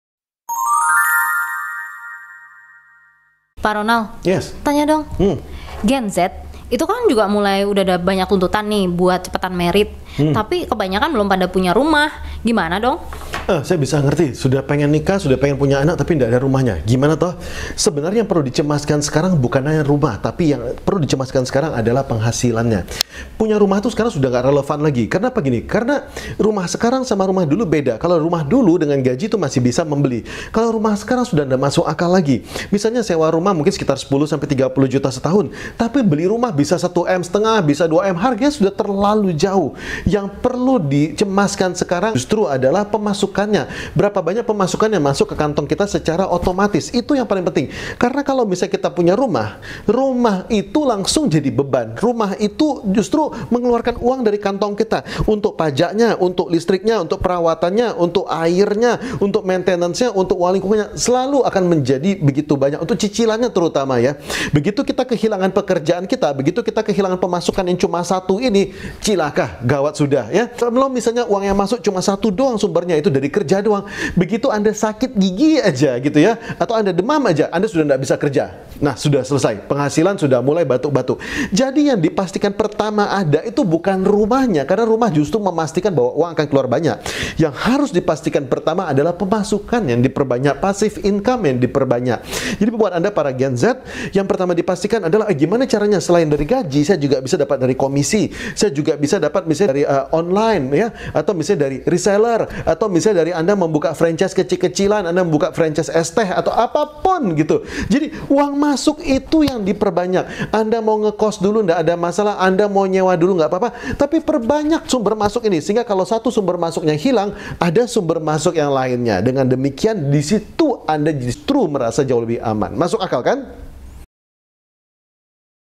GEN Z Harus Sudah Punya Rumah
Gen Z, yang mulai ditanya-tanya buat nikah,

Apakah harusnya udah mulai mikirin punya rumah, seperti generasi2 sebelumnya?

Kalau belum punya rumah harus bagaimana?
Simak video ini untuk temukan jawabannya.

Ingin mengundang, hubungi Renny di: + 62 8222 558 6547